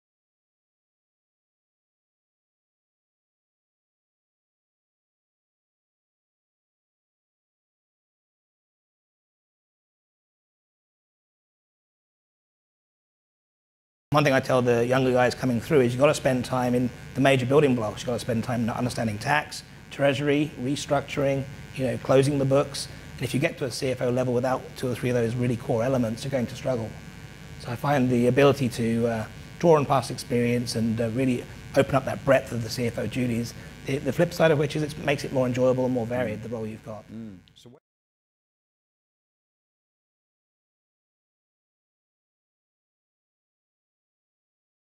The Modern CFO Skill Set
Richard Rogers, CFO Lenovo ANZ, shares his take on the modern CFO and what his teams capabilities need to be to achieve a CFO role